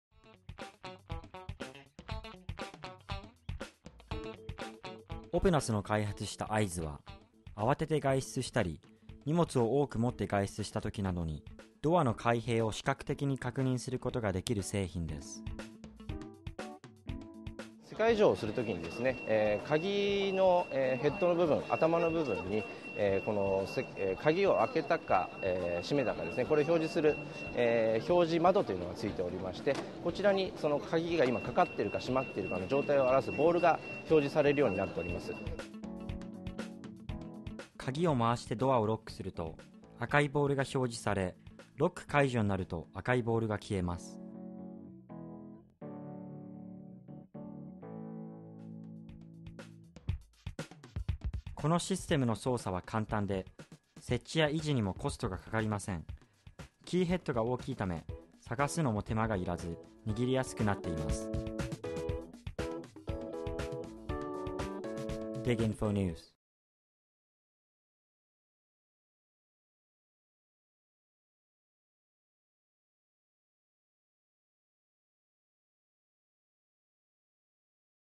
施解鍵状態表示キーアイズ : DigInfo
オプナスの開発したeyeZU（アイズ）は、あわてて外出したり、荷物を多く持って外出したりしたときなどに、ドアの開閉を視覚的に確認することができる製品です。
カギを回してドアをロックすると　赤いボールが表示されロック解除になると赤いボールが消えます。このシステムの操作は簡単で、設置や維持にもコストがかかりません。キーヘッドが大きいため、探すのも手間が要らず、握りやすくなっています。